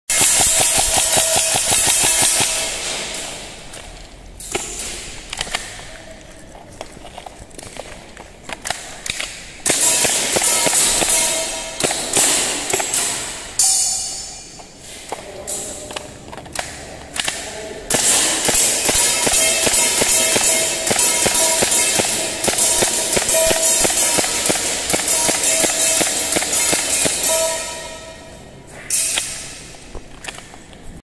OP4@Tokku: Airsoft Mag Dump Picture in Picture Test
I mess with some picture and picture using some footage from my GoPro and Lucky's phone. I almost look semi-competent!